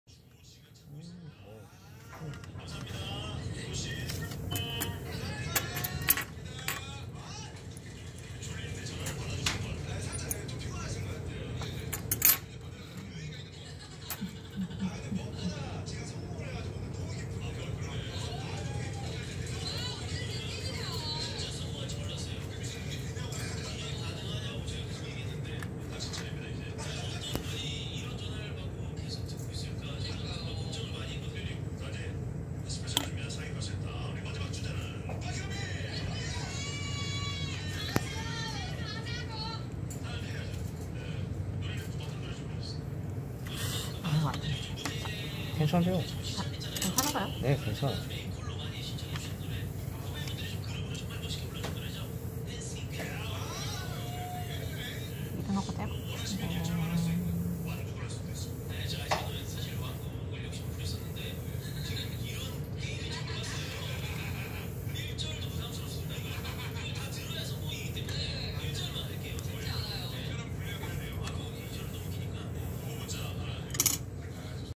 튜닝타임즈 강좌 명동 정화예술대학교 평생교육원 OOO님의 Tamiya Custom 타미야 커스텀 1/48 M4 Sherman 셔먼 데칼 점착 작업
명동 정화예술대학교 평생교육원 프라모델 에어브러시 수업 수강생 OOO님이  평소 작업하고 남았던 데칼들을 활용해서 레이싱머신처럼 화려하게 꾸며가고 있는데요, 지난주엔 수박바처럼 보였지만 화려하게 데칼을 붙여주고 나니 확실히 달라 보이는 자캐가 되어가고 있습니다^^